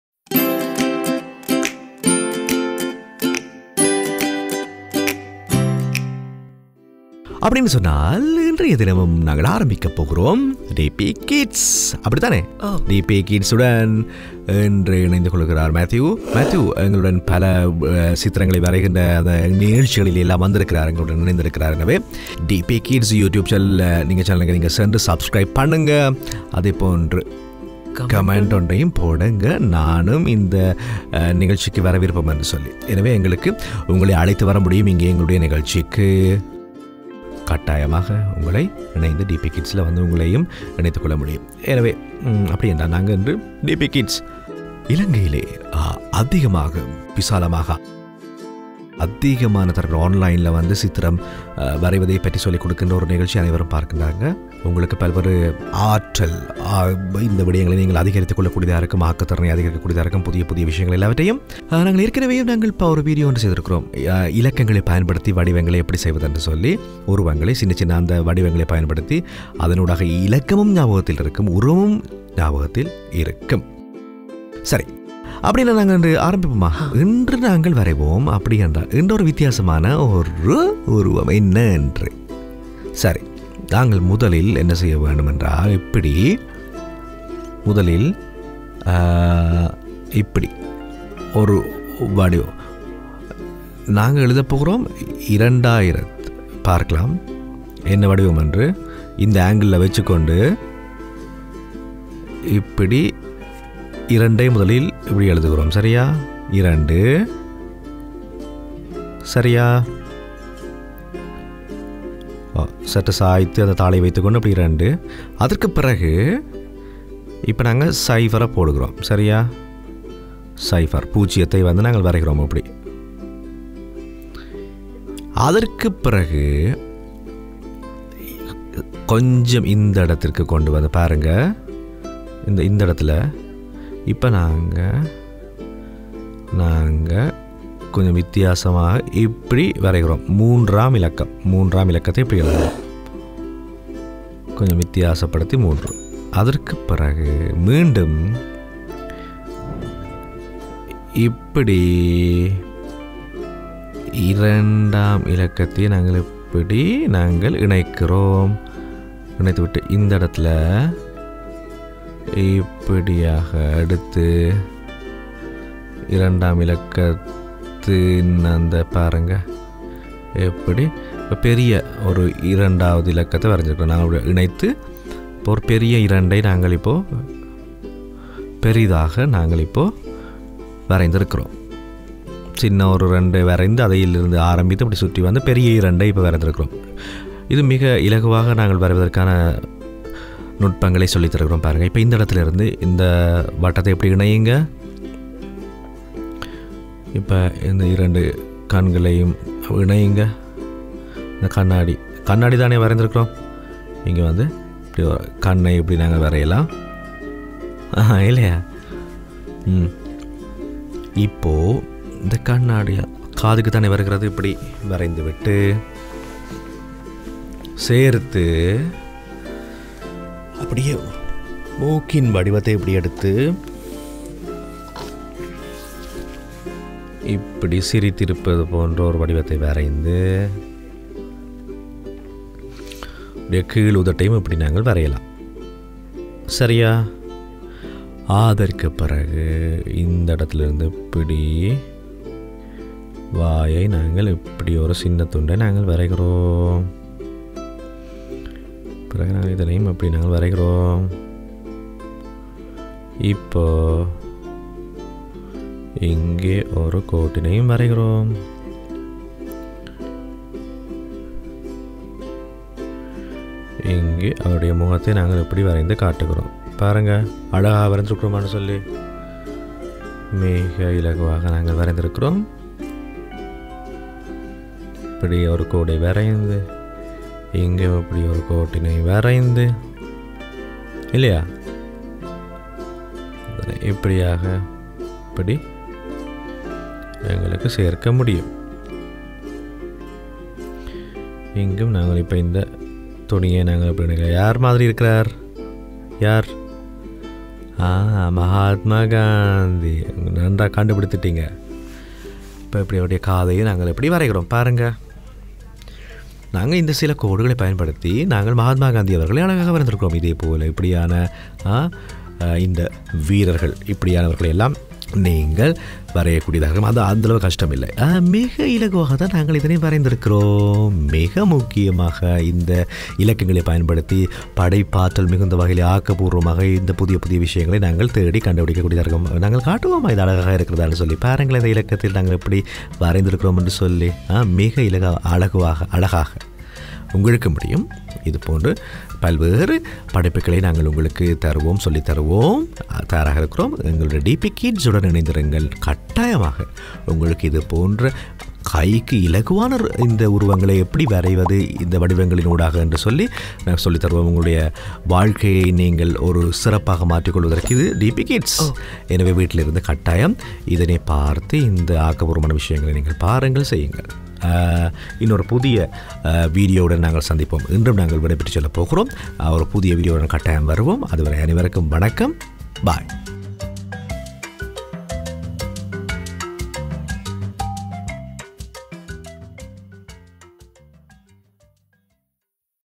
எண்கள் 02 இலிருந்து காந்தியை எப்படி வரையலாம் - டிபி கிட்ஸ் வரைதல் மற்றும் கைவினை (வீடியோ 485)
▶️ இந்த காணொளியில் எண்கள் 02ல் இருந்து காந்தியை எப்படி வரைவது என்று பார்ப்போம்
▶️ In this video we will see how to draw a Gandhi from numbers 02
TIMESTAMPS:
அறிமுகம் - 0:00
எண்கள் 02 இலிருந்து காந்தியை எப்படி வரையலாம்  -  1:40